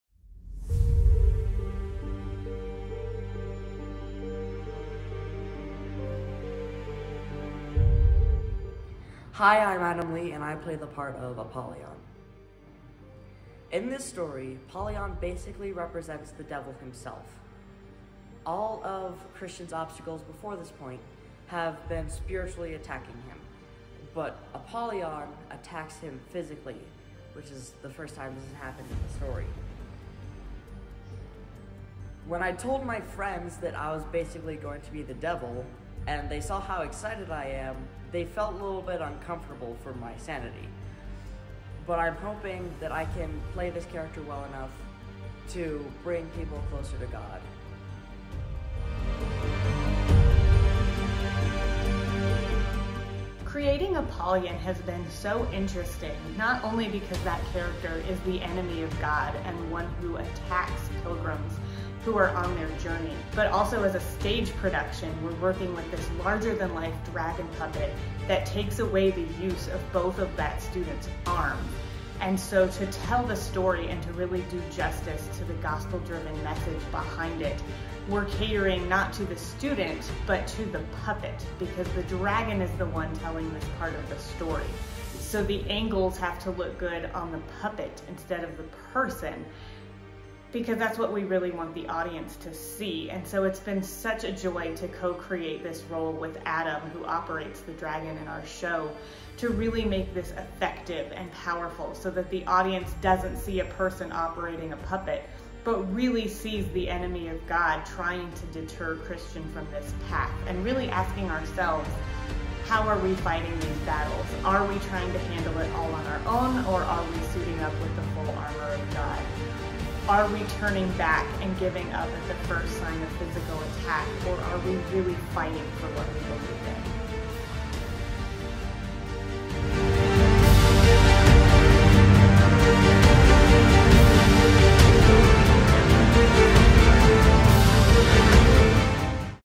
Creating Apollyon
He's the guardian of the abyss. He's the opposite of spiritual freedom. He's the enemy of God. Oh, and he's a giant and powerful dragon. See how we're bringing Apollyon to life this season, then come see for yourself live on stage this November. Tickets to "The Pilgrim's Progress" go on sale October 2022!